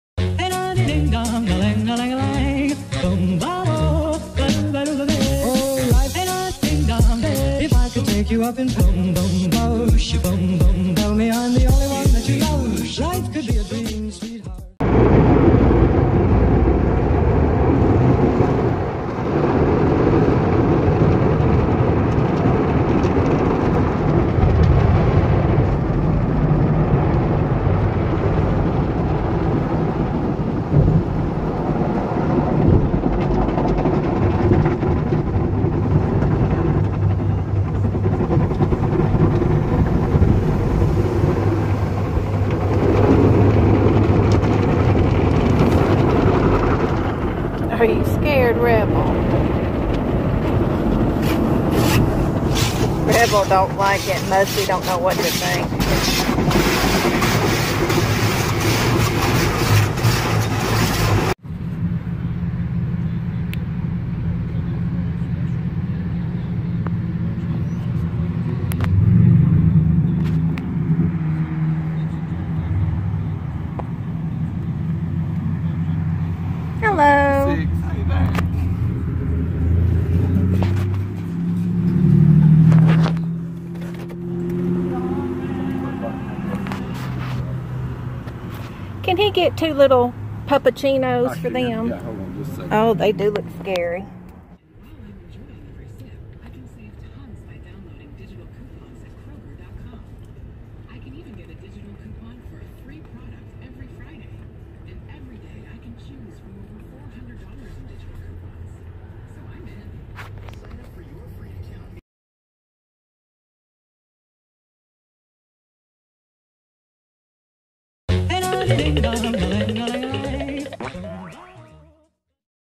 A Day In The Life Of Mosby And Zombie Frappuccinos
Mosby had a busy day at the car wash and picking up treats. He gets into all sorts of adventures while his Daddy is at work. Will doesn't believe all the things Mosby gets into.  Today he washed my car and tried the new Zombie Frappuccinos at Starbucks. It was delicious but very high calorie.